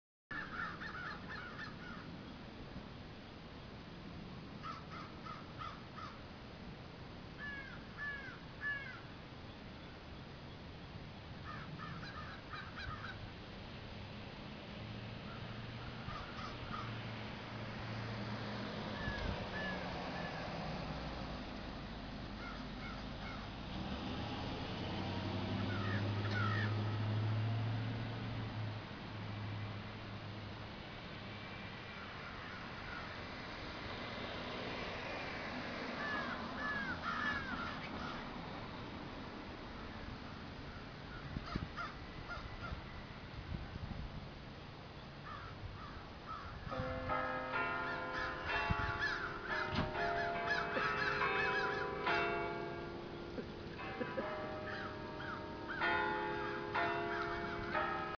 Crows get a surprise at the top of the hour.
Heard a mob of crows on top of the carillon and went to investigate. Had a chuckle when the hour struck.